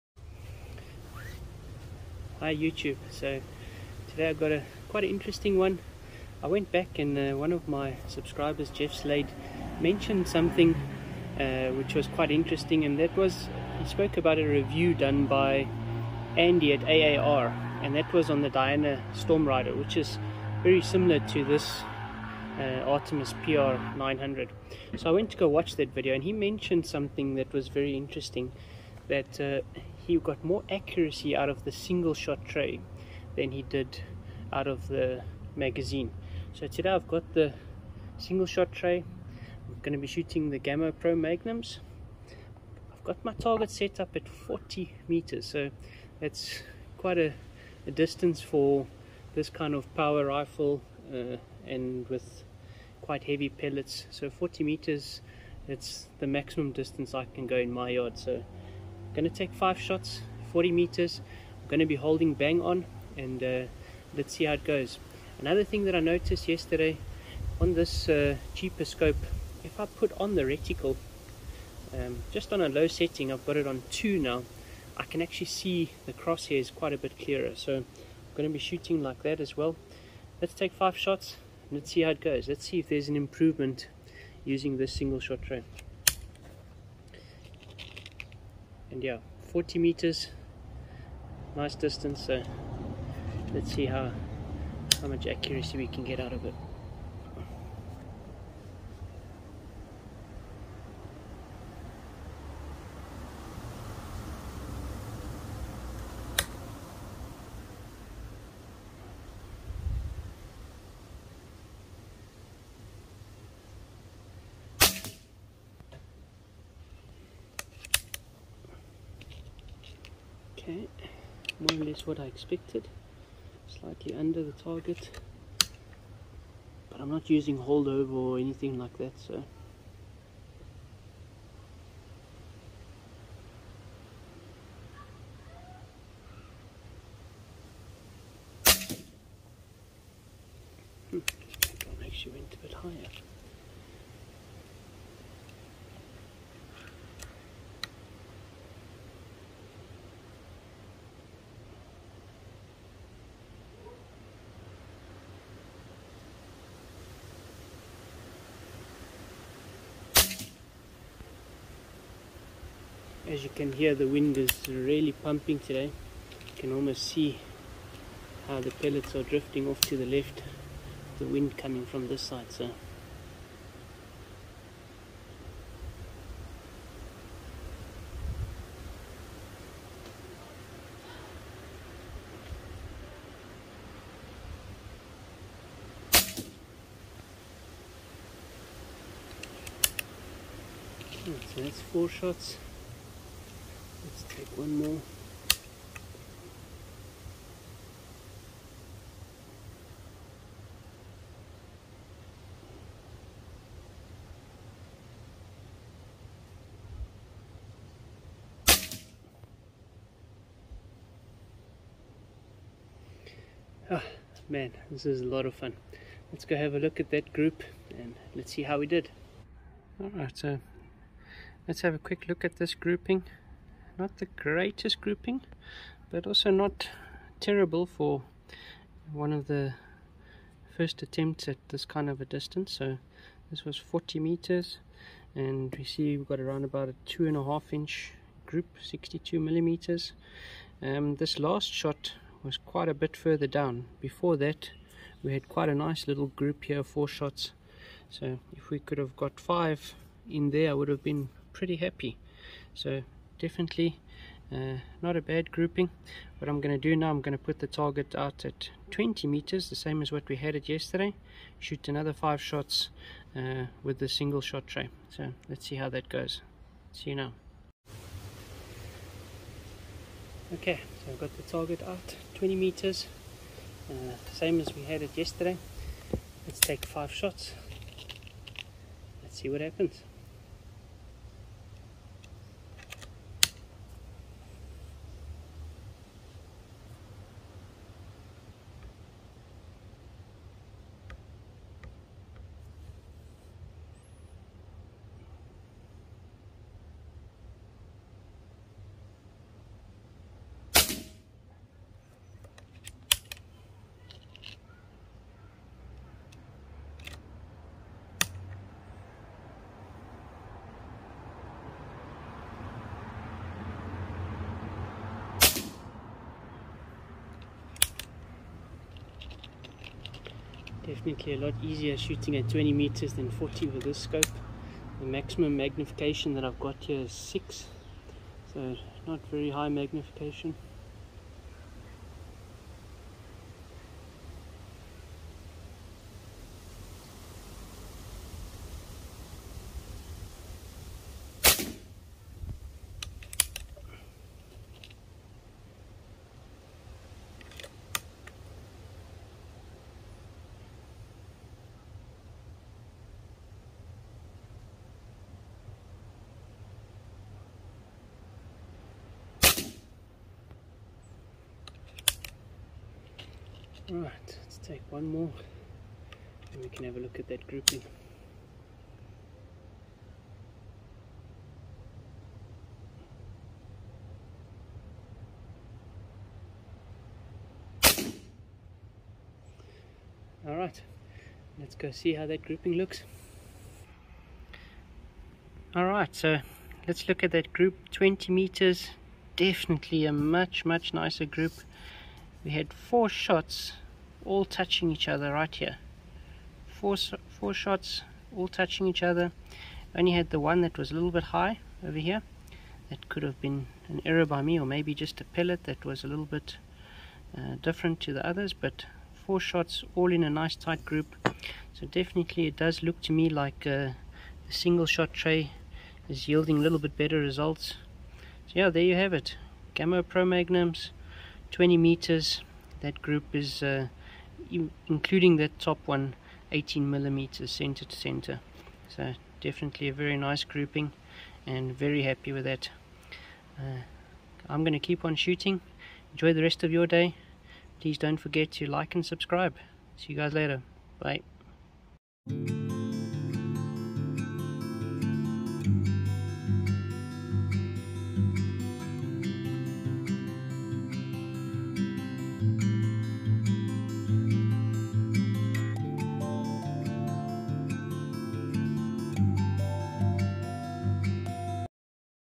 Artemis PR900W - Single shot tray vs Magazine testing
following on yesterday's accuracy testing I wanted to see if there would also be a difference between the single shot tray and the magazine, also did some distance testing up to  40m

If you would like to contribute to the channel please use my Paypal tip account

If you love your CP2 as much as I do remember to check out this playlist

Happy Shooting!